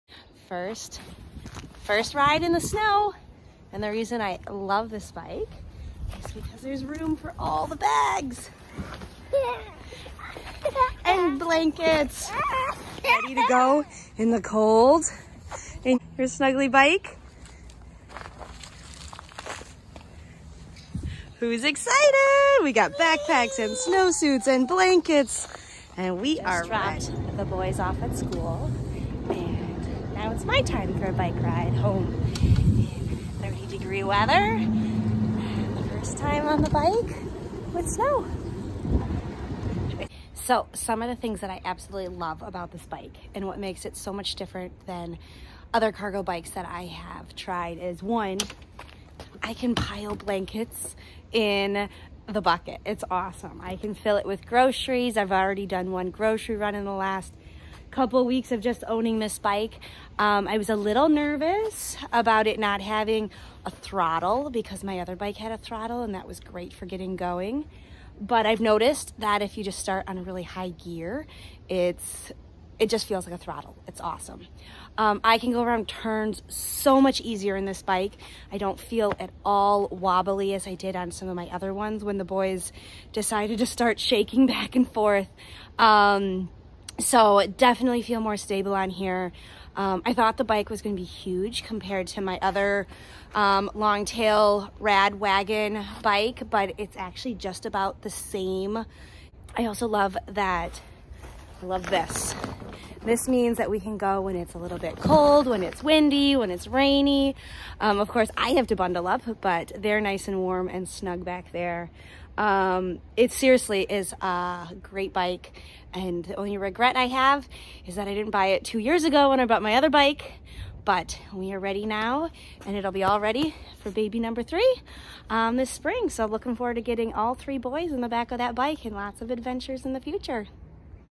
Summer Leeper Video Review in Madison, Wisconsin
Summer gives a synopsis of her MADSEN bucket bike, a few weeks after receiving it and during the first snow ride in Madison, Wisconsin.